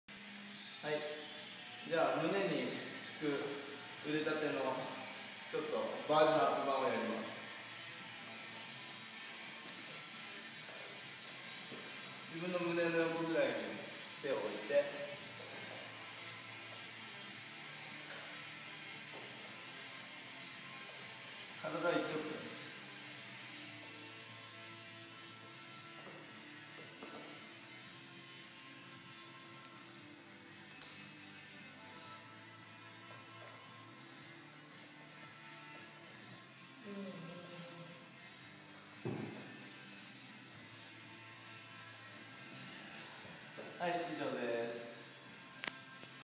【筋トレ】プッシュアップバーで胸を鍛える腕立て伏せ
プッシュアップバーを使う事で、より高い負荷をかける事ができます。胸の横に手をつき、体を常に一直線に保ちながら、地面スレスレまであごを下ろします。

【あなた専用の筋トレメニューを動画にして指導します】